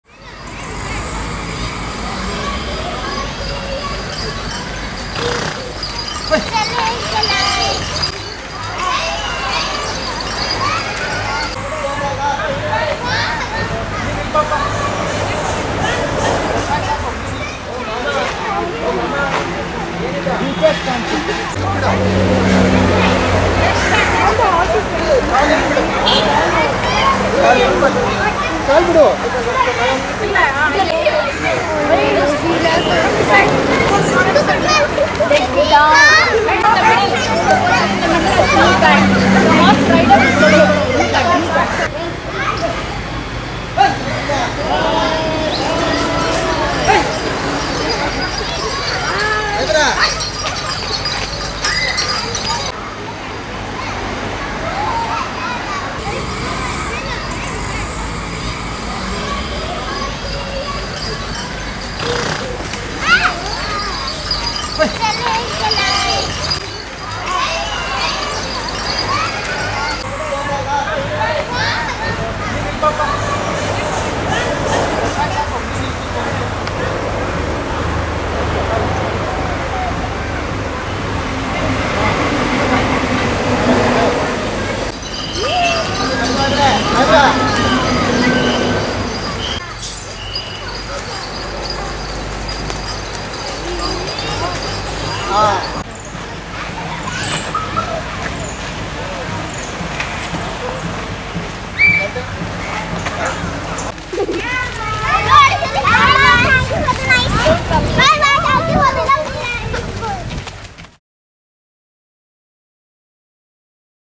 Tanga ride for kids in Bangalore, Karnataka
Small kids take ride on tanga or horse bullock at a garden in Bangalore, Karnataka.